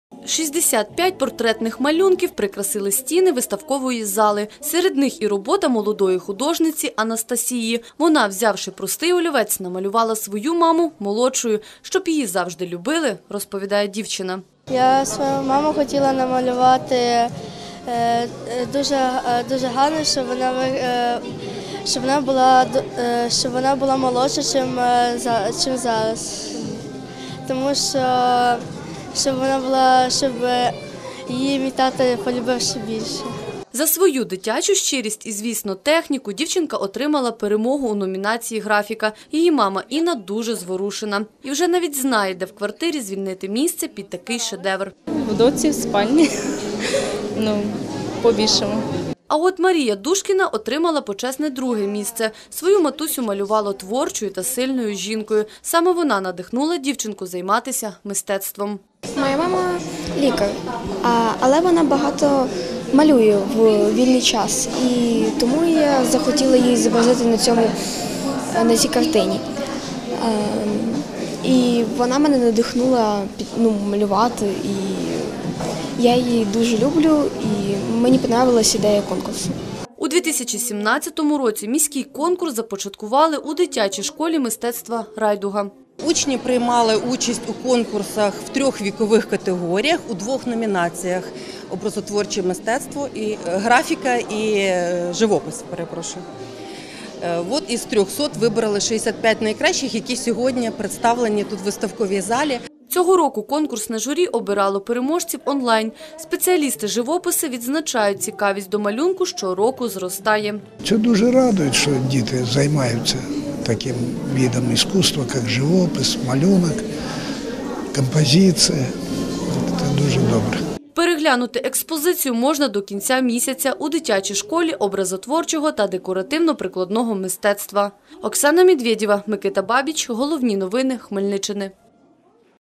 65 портретів матусь презентували діти
Особливі, працьовиті та найкращі друзі – такими малювали своїх найрідніших матусь учасники міського конкурсу малюнку. Його організовують вже вдруге з нагоди святкування Дня матері. На розгляд журі подали понад 300 живописних малюнки, з них 65 увійшли до експозиції.